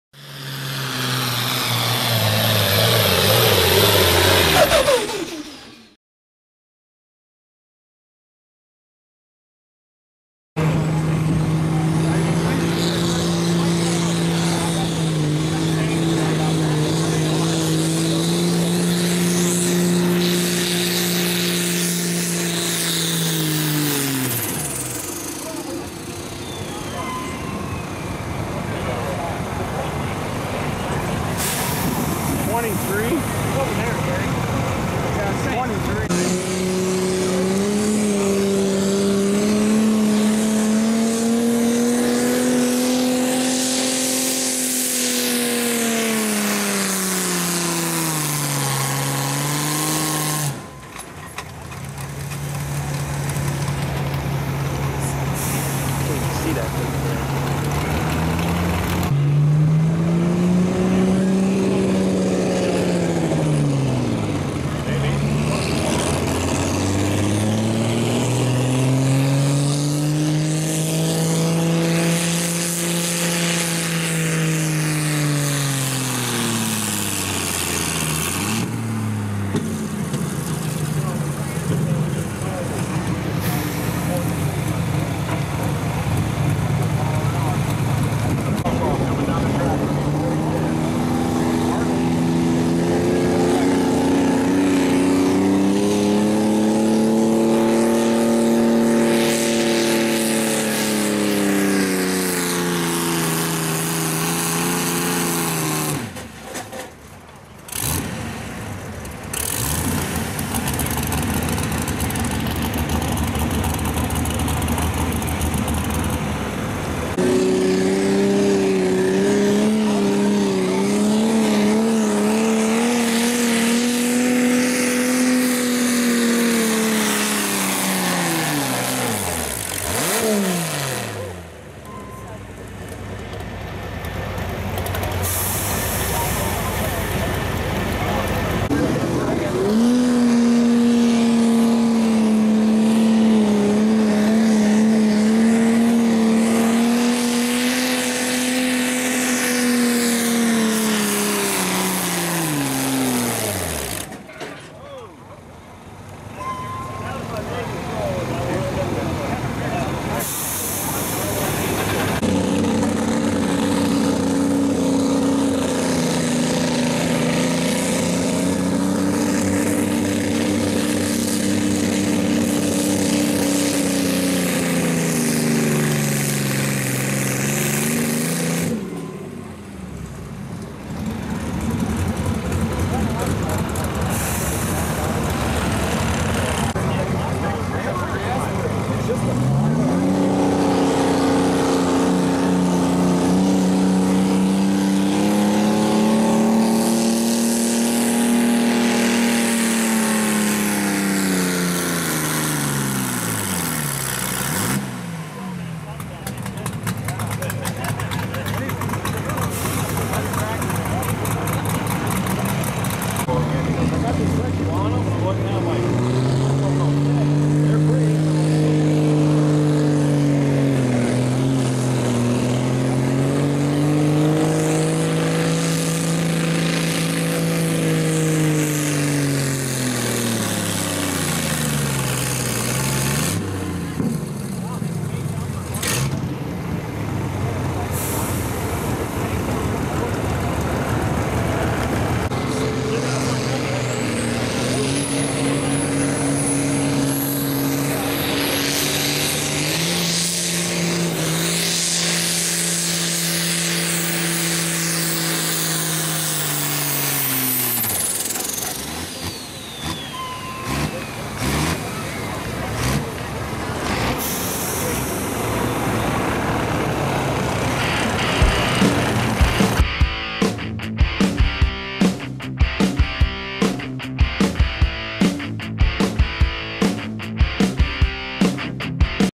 ISP 7,000lb NA Super Stock Tractors in Sheffield, IL July 8, 2017
Make sure to like Plowboy68 Pulling Videos on Facebook!!